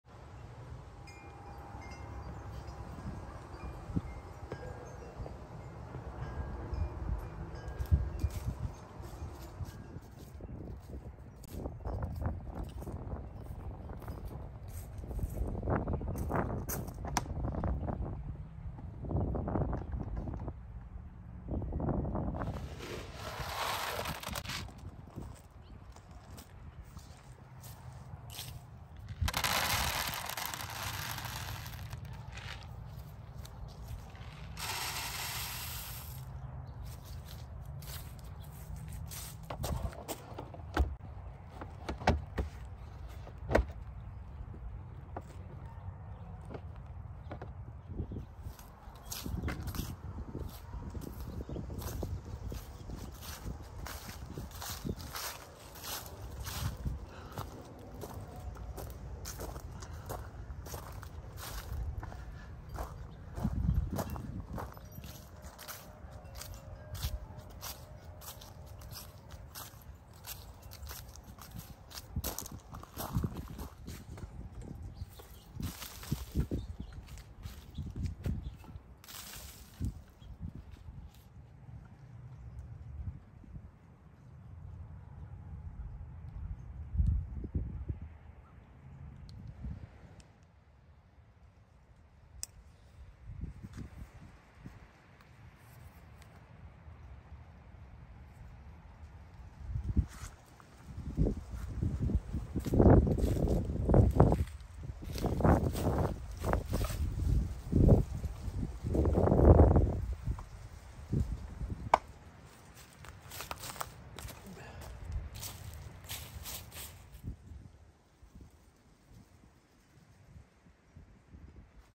Feeding Deer Corn and Bird Feeders
Feeding corn to our local deer.  It keeps them back in the woods and off the roads.  My birds feed on about a half gallon of millet, cracked corn, and sunflower seeds.  Weather today was 62 degrees for December 27, 2021.  When the temps drop,  the birds eat more.